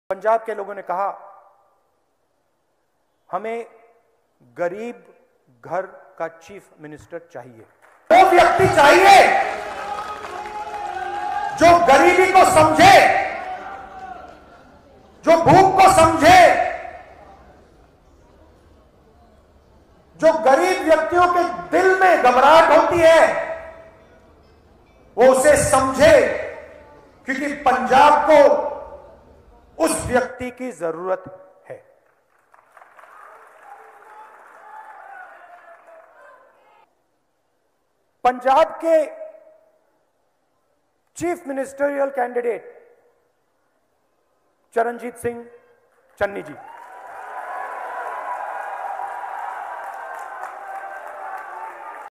Breaking: Charanjit Singh Channi Declared As Congress' CM Face For Upcoming Punjab Polls
Charanjit Singh Channi will be the Congress' chief ministerial candidate for the upcoming Punjab Assembly elections: Rahul Gandhi in Ludhiana.